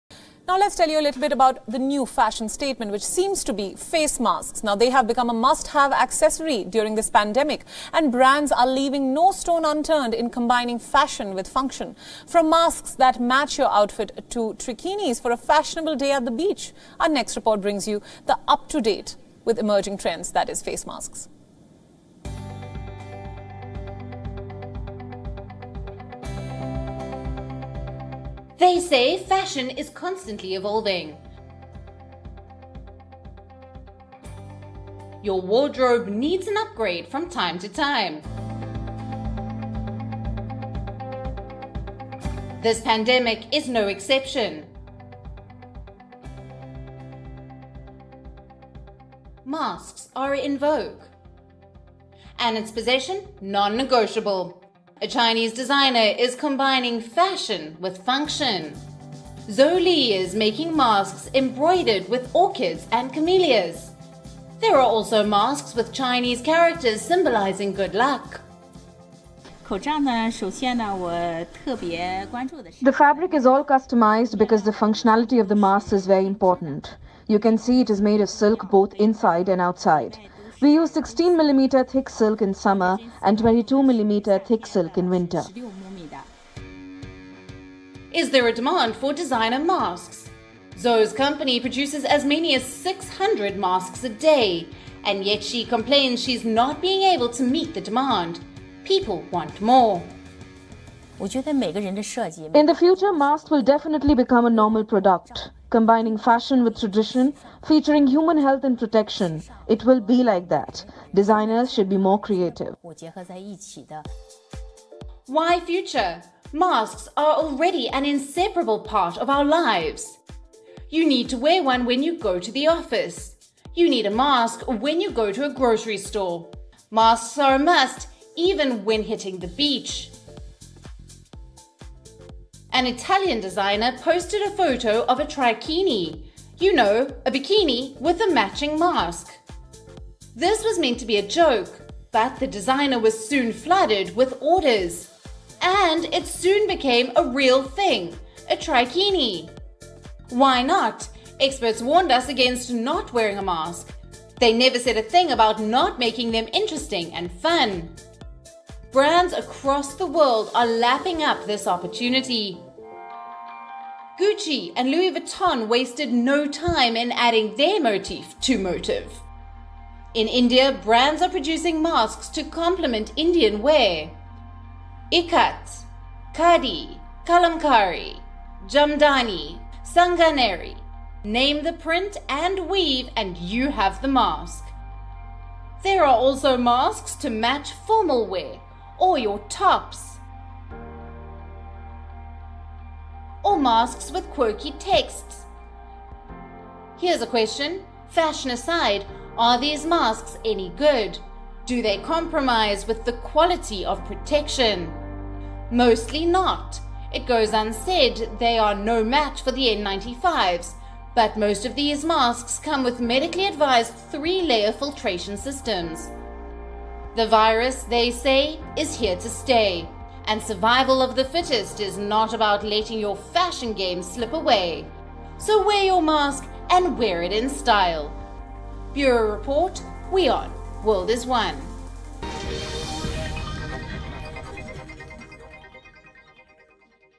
Coronavirus Pandemic Designer Masks in sold in Chinese Market Fashionable Masks
After virus China has now come up with designer mask to protect people from Covid-19.These designer masks are being sold in Beijing market as 16 dollar each.These masks are matches with your outfit. Its made by trikini,a three piece bikini.